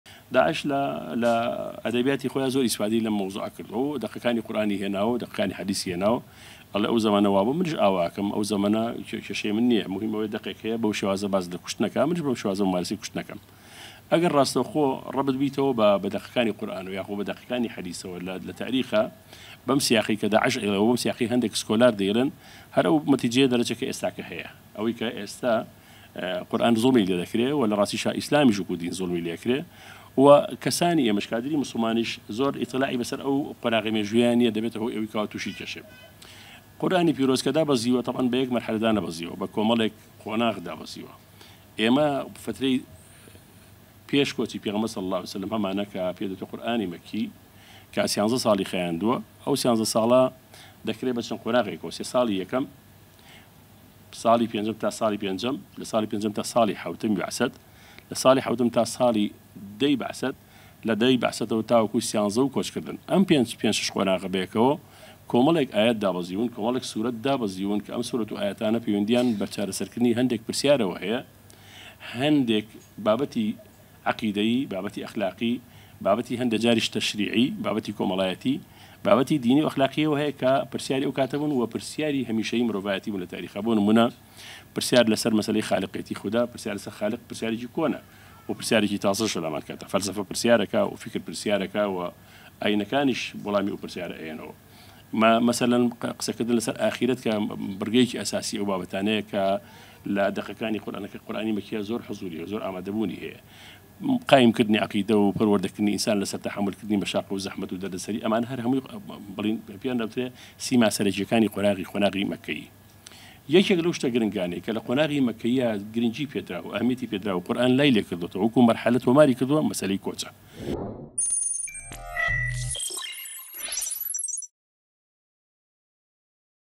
گرنگی زانینی کات و شوێن و بارودۆخی دابەزینی ئایەتەکان
تێنەگەیشتن لە کات و شوێن و هۆکاری دابەزینی ئایەتەکان
هۆکارە بۆ ناشیرینکردنی اسلام لە کاتی جێبەجێکردنی یاساکانی قورئاندا